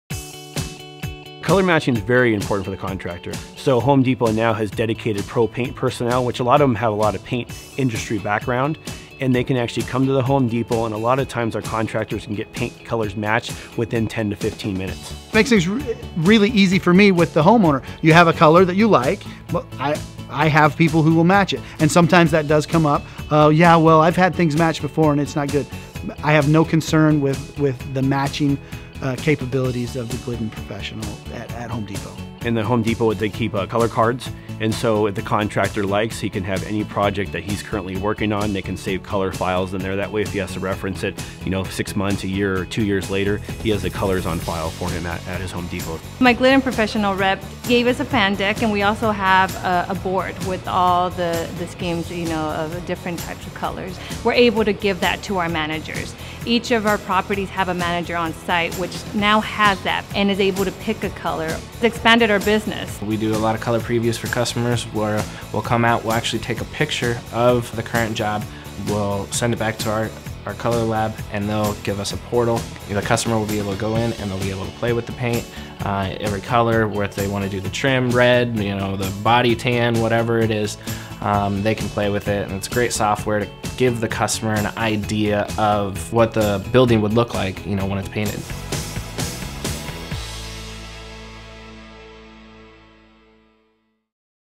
Glidden Professional® Color Services
Customers rely upon Glidden Professional® color services to keep their businesses running smoothly. Hear more about color matching and consistency, custom fan decks and the online Previews® Color Visualization service which allows color options to be viewed prior to painting.